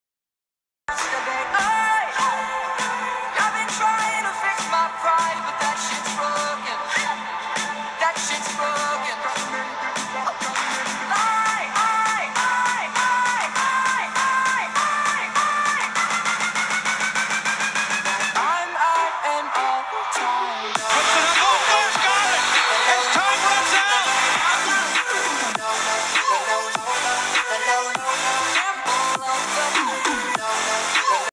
Unbelievable Buzzer Beater!!! How Did That Go In?!?!?!
In the summer league, he makes an outstanding shot to win the game!